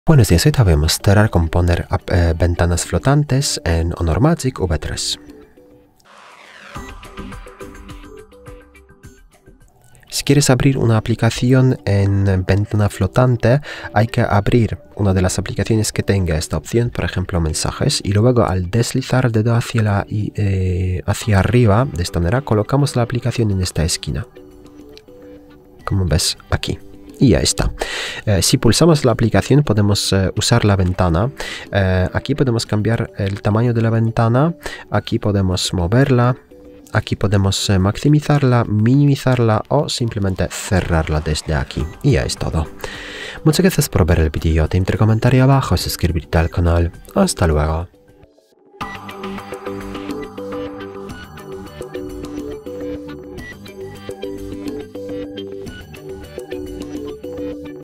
Cómo activar ventanas flotantes en el HONOR Magic V3
¿Quieres maximizar tu productividad en el HONOR Magic V3? En este video, te mostramos cómo activar y usar las ventanas flotantes para que puedas multitask de manera eficiente. Aprende a abrir múltiples aplicaciones al mismo tiempo y disfruta de una experiencia de usuario más fluida y versátil.

¿Cómo usar aplicaciones en ventanas flotantes en HONOR?
¿Qué son las ventanas flotantes en Android?
¿Cómo gestionar ventanas flotantes en el HONOR Magic V3?
¿Cómo cerrar ventanas flotantes en mi HONOR Magic V3?